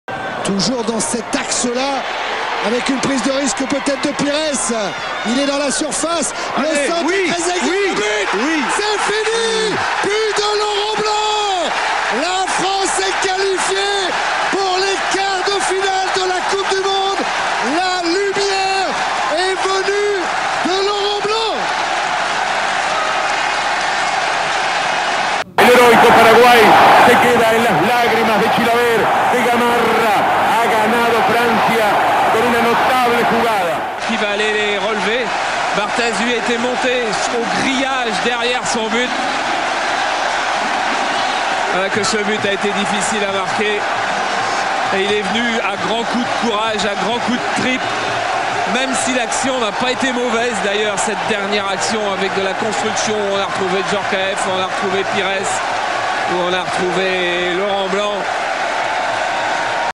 The 1st World Cup Golden Goal! – France 🇨🇵 v Paraguay 🇵🇾 (1998)
Lets remember together how Laurent Blanc broke the resistance of José Luis Chilavert to score the 1st Golden Goal in World Cup history and to beat Paraguay and qualify to the quarterfinals of France 98!